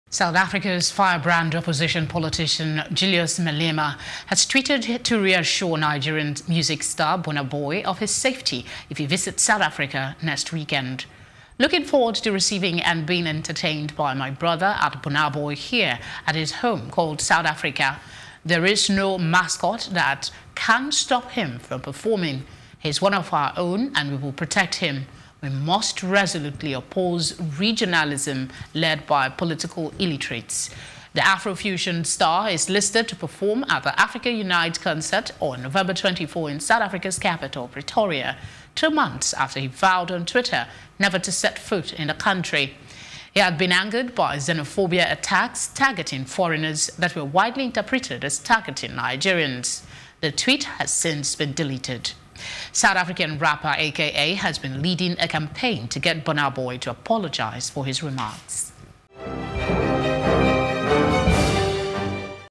South Africa's Trip: Malema Assures Burna Boy Of Protection
South Africa's firebrand opposition politician Julius Malema has tweeted to reassure Nigerian music star Burna Boy of his safety if he visits South Africa next weekend.
Looking forward to receiving and being entertained by my brother @burnaboy here at his home called South Africa. There's no mascot that can stop him from performing, he's one of our own and we will protect him. We must resolutely oppose regionalism led by political illiterates.
The Afro-fusion star is listed to perform at the Africa Unite concert on 24 November in South Africa's capital, Pretoria, two months after he vowed on Twitter never to set foot in the country.
He had been angered by xenophobic attacks targeting foreigners, that were widely interpreted as targeting Nigerians. The tweet has since been deleted.

South African rapper AKA has been leading a campaign to get Burna Boy to apologize for his remarks.